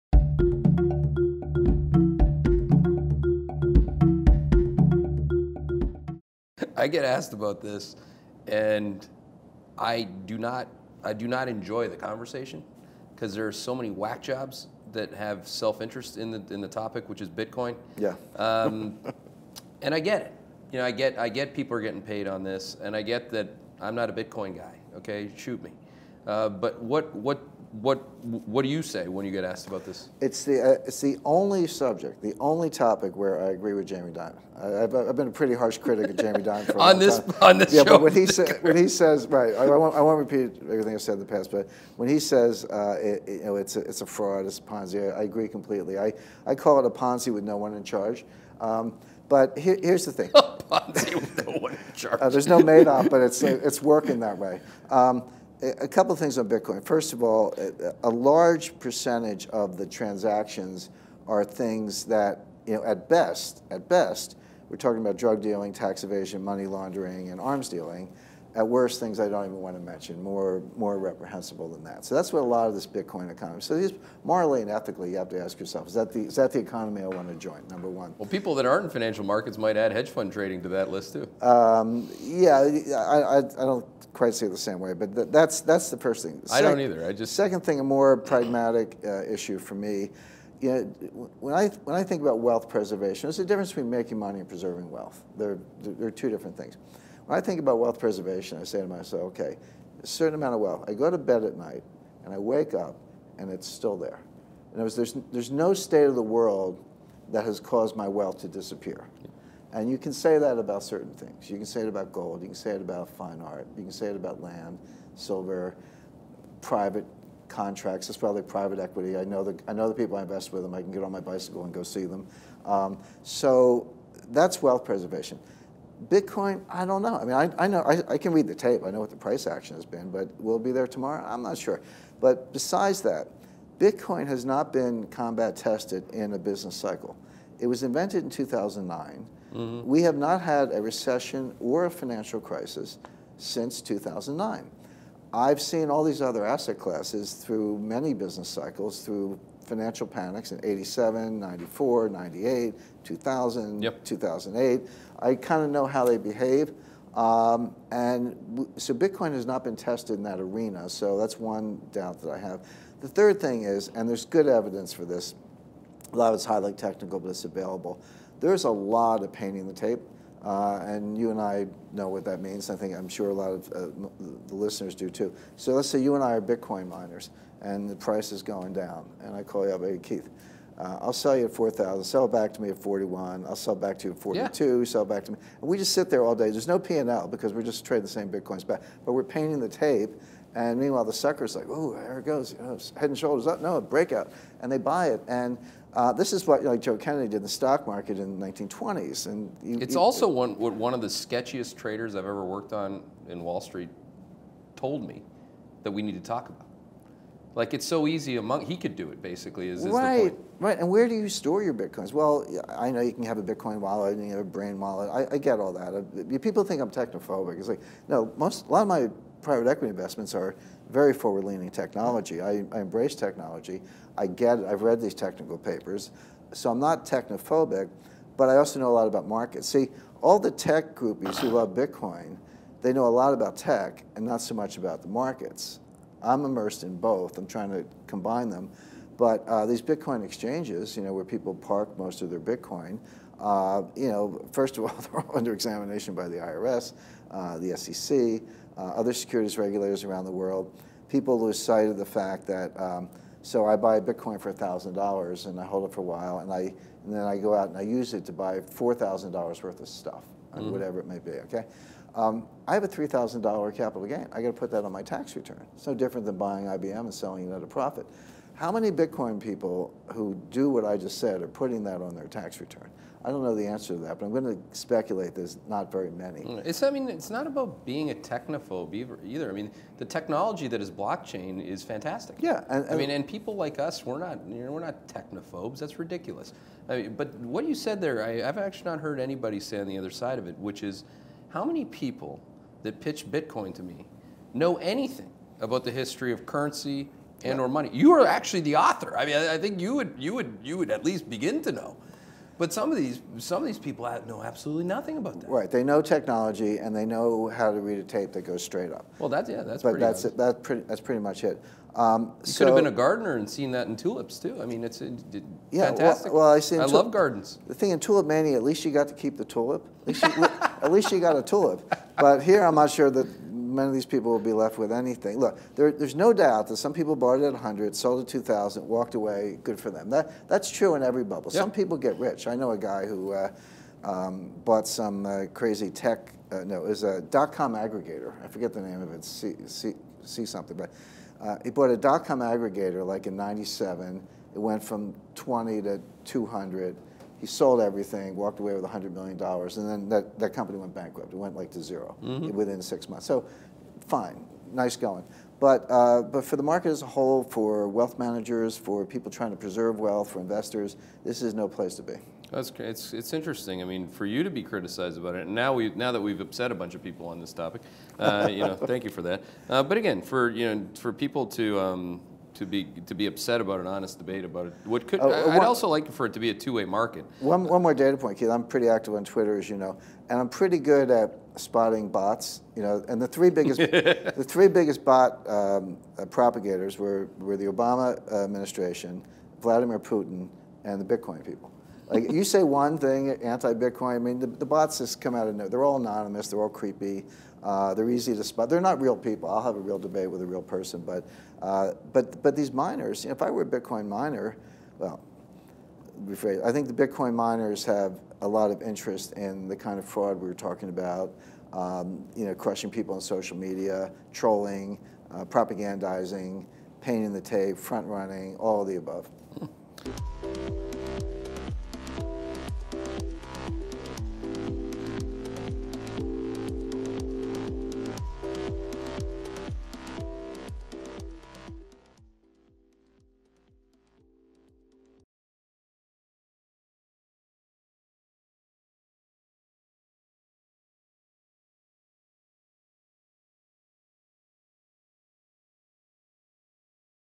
Rickards: ‘Bitcoin is a Ponzi Scheme’
Bitcoin has been in existence for all of eight years but a debate is raging in the investing community: Is the cryptocurrency actually useful beyond just being a vehicle for speculation? At its worst, does it help facilitate highly questionable and illegal transactions and activity?

JPMorgan CEO Jamie Dimon recently called bitcoin a “fraud” that “won’t end well.” The cryptocurrency is up 800% in the past year. So bitcoin investors can have a good laugh at Mr. Dimon’s expense, for now.

Important questions and skepticism remain. Among the cryptocurrency's big  skeptics is currency guru and bestselling author Jim Rickards. He sat down to discuss this (and much more) during a new "Real Conversation" with Hedgeye CEO Keith McCullough.

Rickards begins:

“This is the only topic where I agree with Jamie Dimon. I've been a pretty harsh critic of Jamie Dimon. But when he says 'it’s a fraud, it's a Ponzi'[ scheme]'-- I agree completely. I call it a Ponzi with no one in charge. There's no Madoff, but it's working that way.”

While bitcoin has its fervent supporters, Rickards says the cryptocurrency invented in 2009 still has too much to prove, and is involved in too many shady transactions to be taken seriously. He cites things like drug dealing, arms dealing, money laundering and tax evasion, as well as "worse things that I don't want to even mention, more reprehensible than that."

He goes on further:

“Bitcoin has not been combat tested in a business cycle. We have not had a recession or a financial crisis since 2009. I’ve seen all these other asset classes go through many business cycles. I know how they’ll behave. Bitcoin has not been tested in that arena.”

Rickards and McCullough both say they have both been accused of being "technophobic" because of their distaste for bitcoin. Rickards says that couldn’t be further from the truth.

“A lot of my private equity investments are very forward-leaning in technology. I embrace technology. But I know a lot about markets. A lot of the tech groupies who love bitcoin know a lot about tech, but not so much about the markets.”

Until proven otherwise, Rickards says investors should stay away.

“For the market as a whole, for wealth managers, for people trying to preserve wealth, for investors, this is no place to be,” Rickards says.

Watch the full clip above for more.